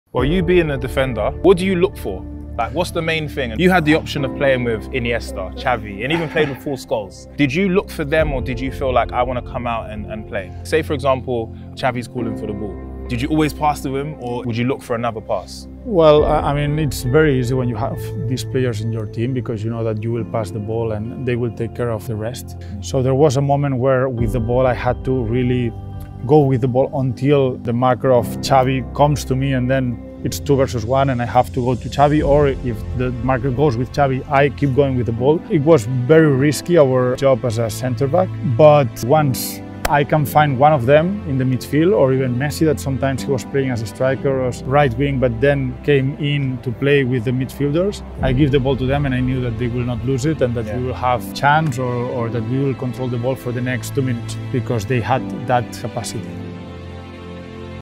Pique on Playing With Xavi Iniesta and Messi
Motivational Video | Motivational Speech | Inspirational Video | Life Success

Speaker: Gerard Pique

just enjoy and share.

Source:
Gerard Pique On Messi, Sir Alex Ferguson & Pep Guardiola | Barcelona To Kings League Ft Harry Pinero